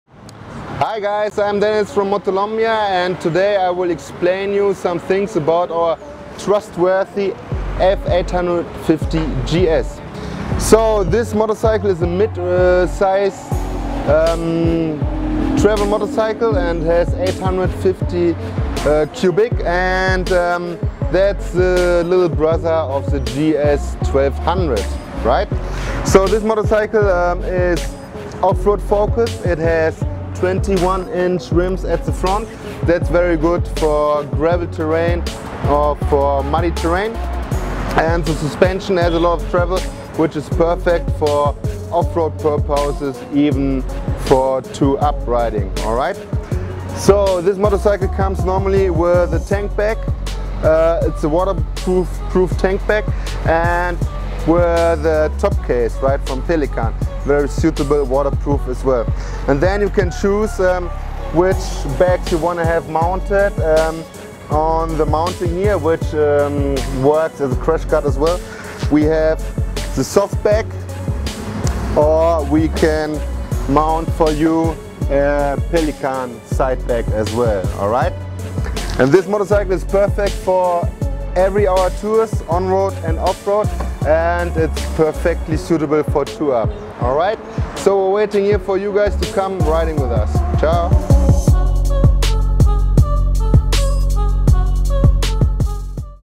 What to Expect? Rent a BMW F850GS from Motolombia in Colombia
- Check out the set-up of our rental BMW F850GS bikes. Come ride with Colombia's most experienced tour and rental operator Motolombia!